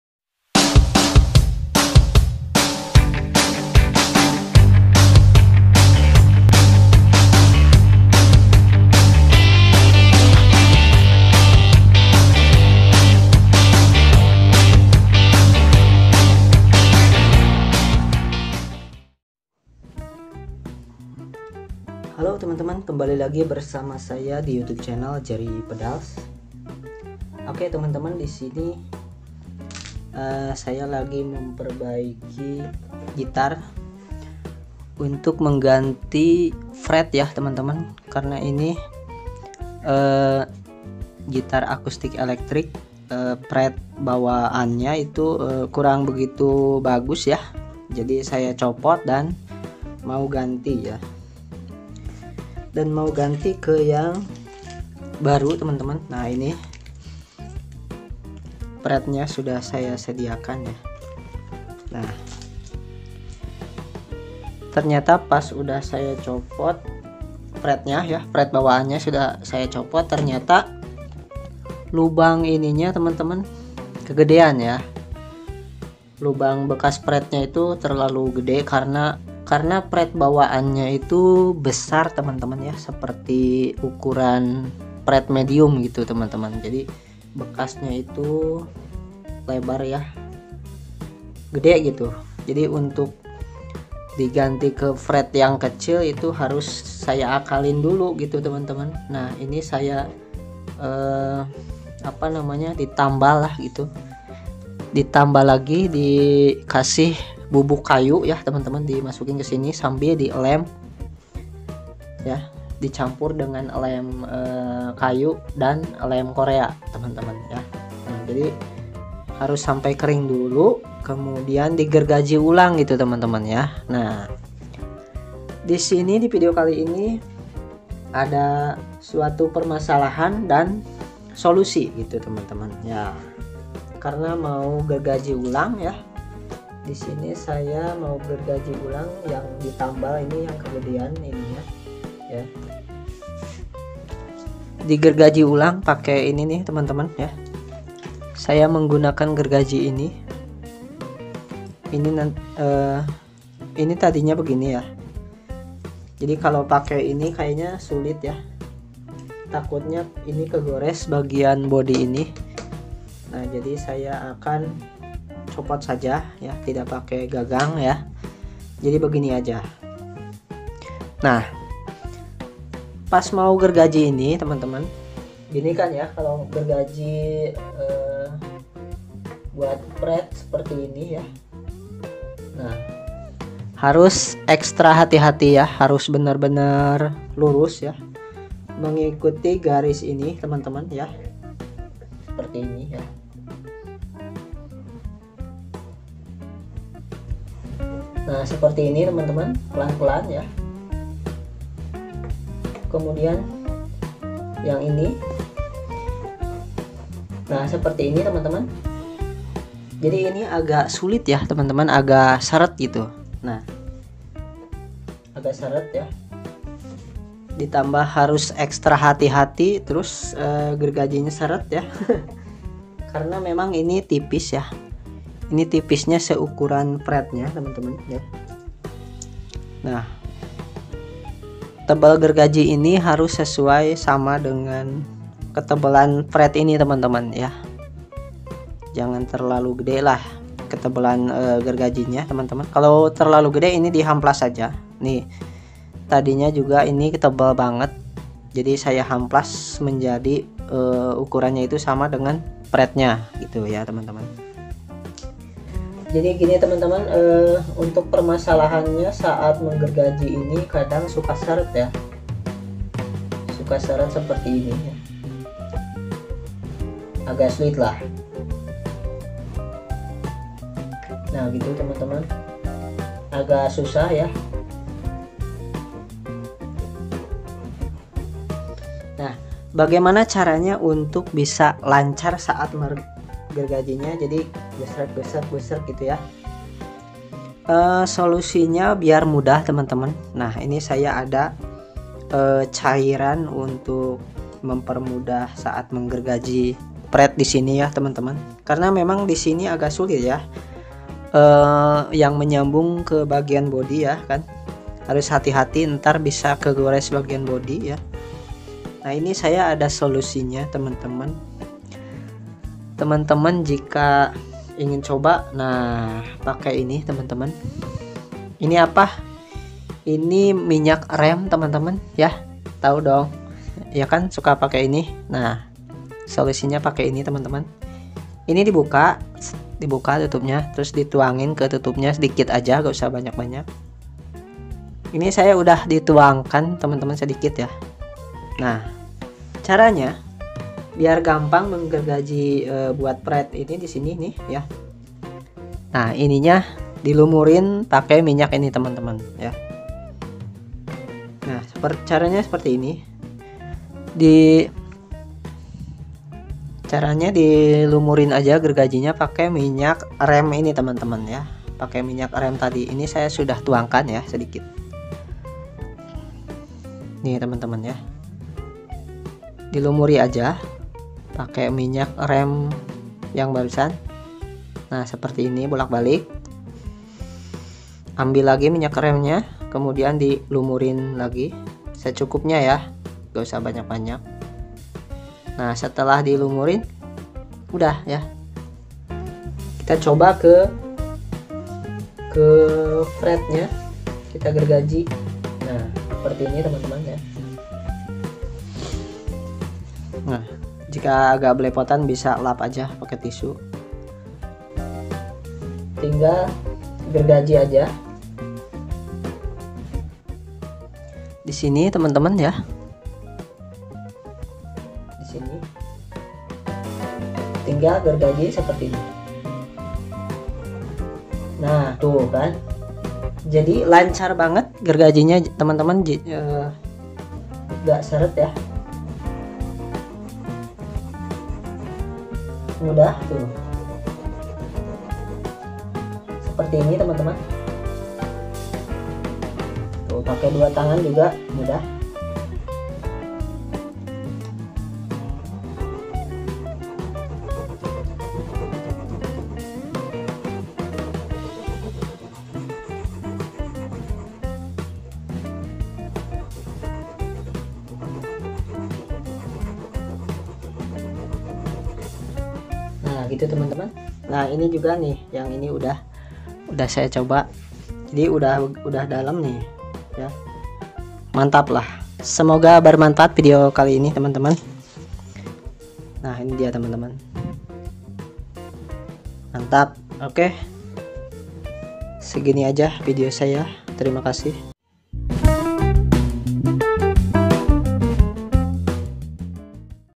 Cara Memperbaiki Fingerboard Fret Gitar Akustik
di video ini saya melepas fret gitar akustik yamaha APX 500ii pemula, dan memperbaiki fingerboard fret nya yang kurang presisi